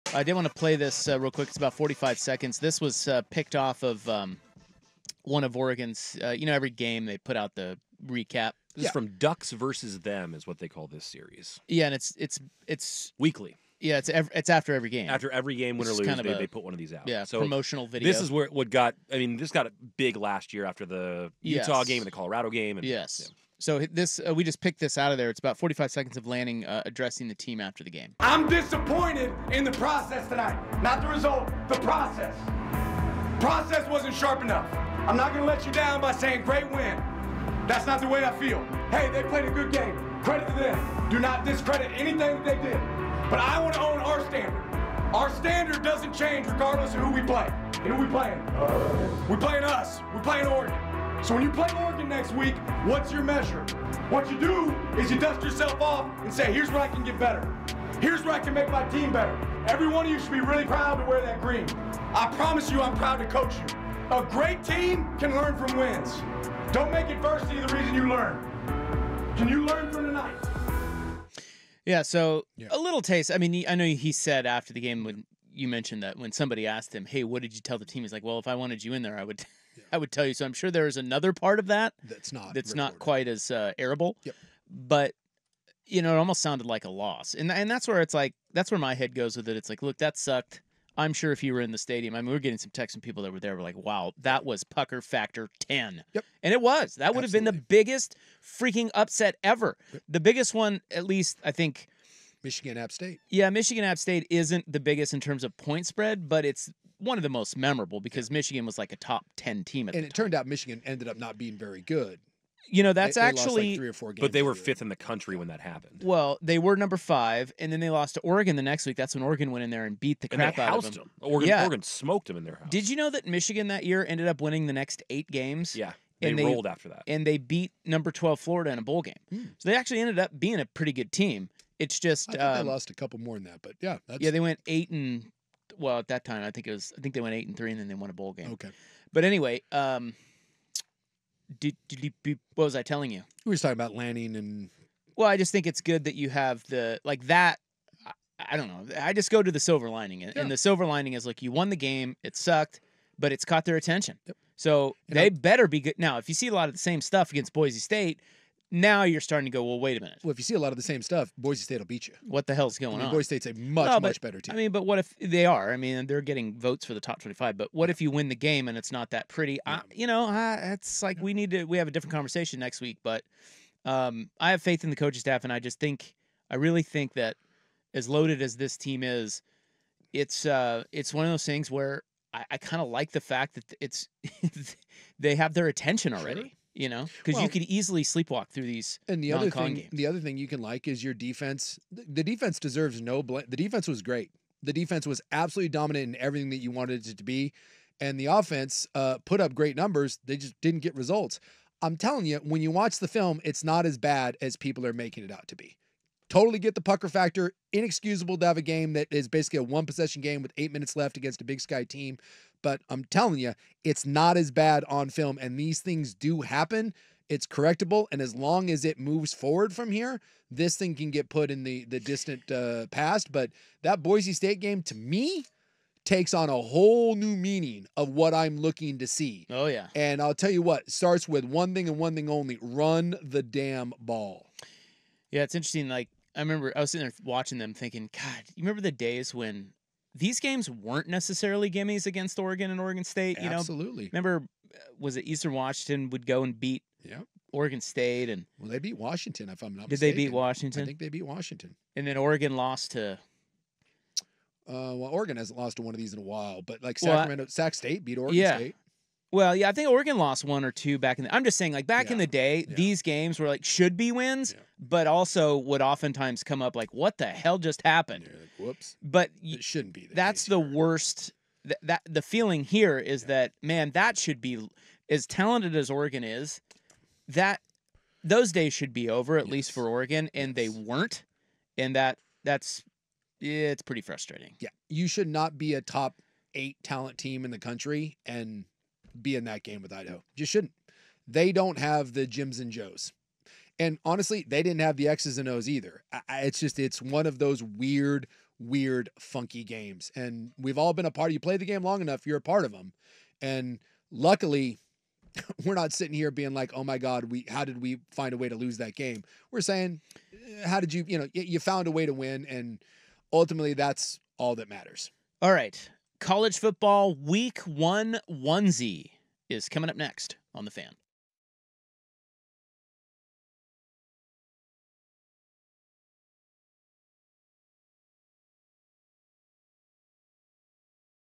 How Does Oregon Move Forward From Nearly Losing To Idaho? | Primetime W/ Isaac & Suke
After nearly losing to an FCS team, there are a lot of things that Oregon needs to do if they want to meet the expectations for them this year.